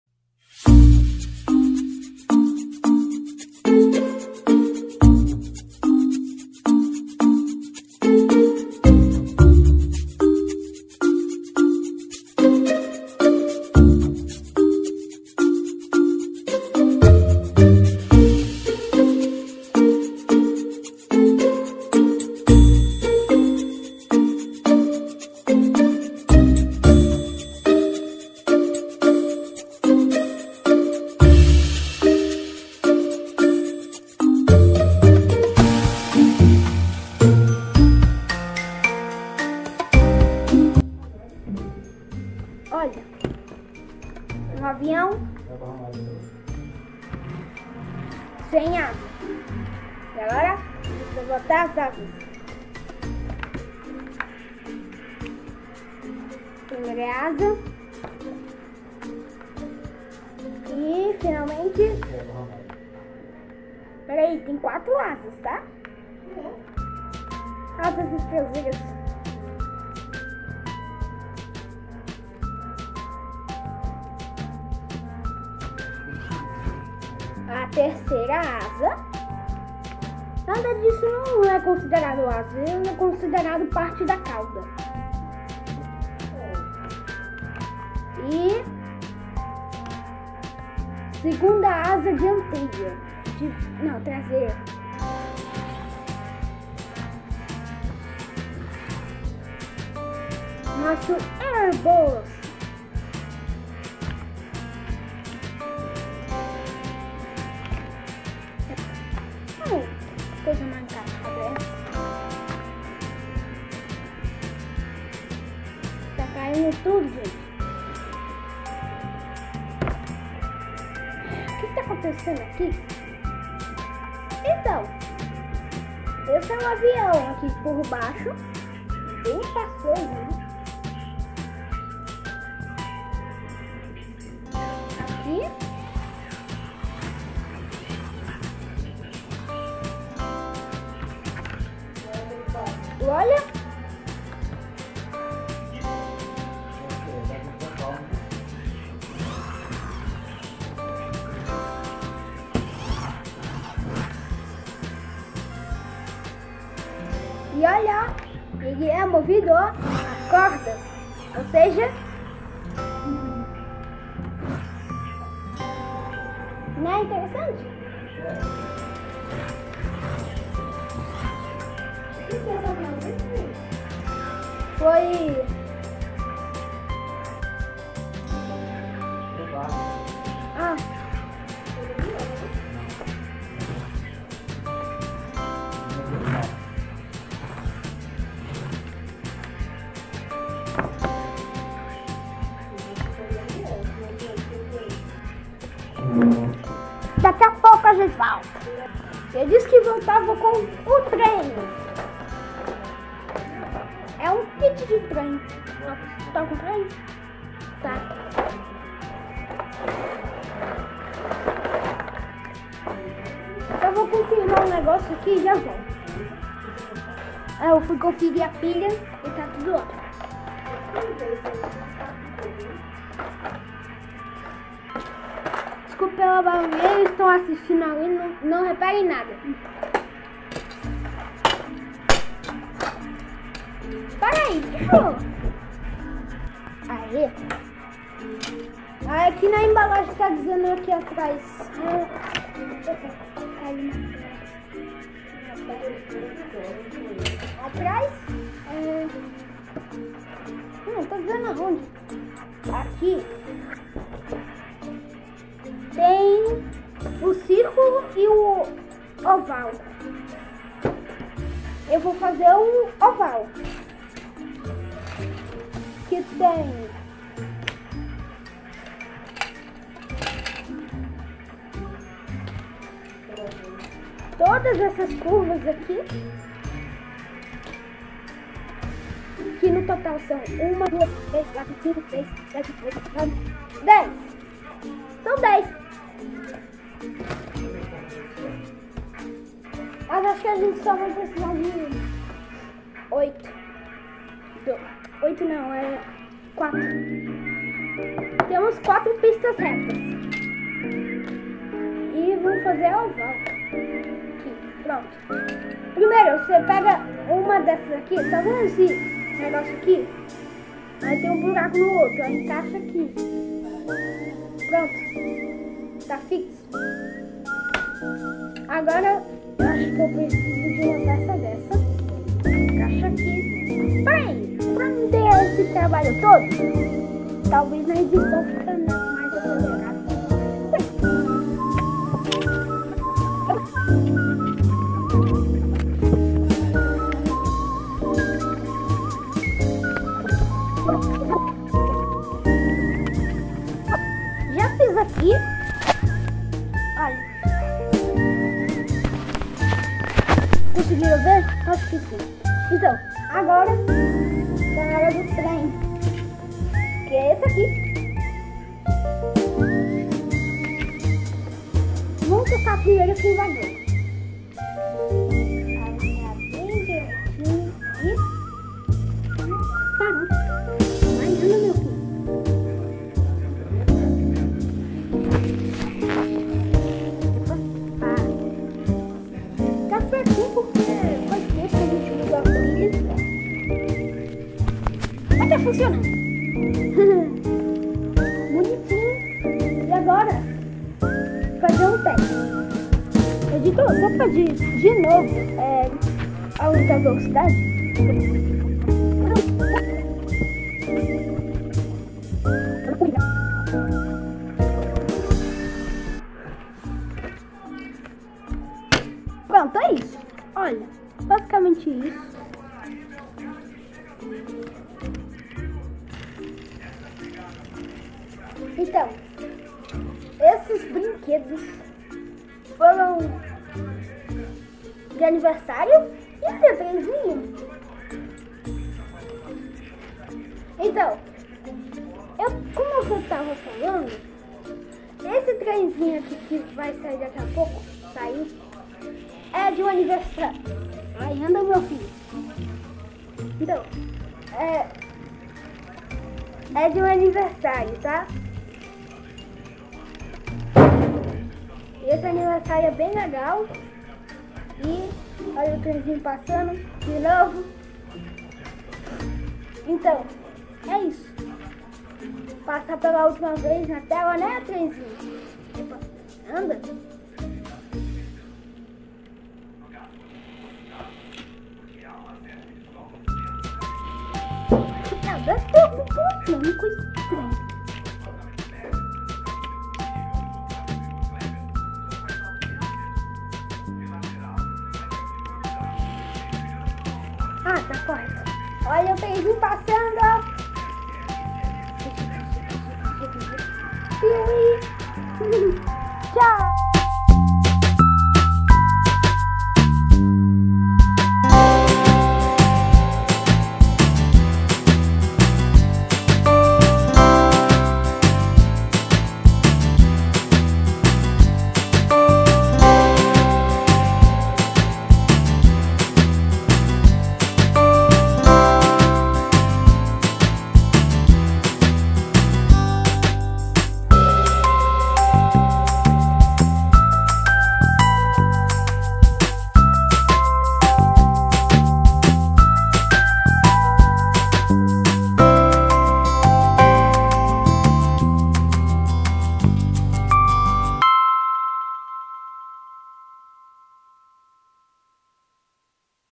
UMBOXING DUPLO!, Airbus e Rail King no mesmo vídeo, e muito mais... / LEVY EMANUEL.
Airbus e Rail King vem nessa jornada dos itens da família OLIVEIRA! Por que, recentemente, postei mais Shorts do que vídeos? Porque eu quero! kkkkkk

dá uma socada nos botões de inscrição e like, depois, acesse nosso Kawaii: Levy Oliveira7473 que você vai gostar!

para quem quiser comprar esses brinquedos, estão o nome deles aí em baixo👇

avião=air bus,

trem=rail King.

MOMENTOS IMPORTANTES:
0:00 | início
0:39 | Airbus
3:18 | rail King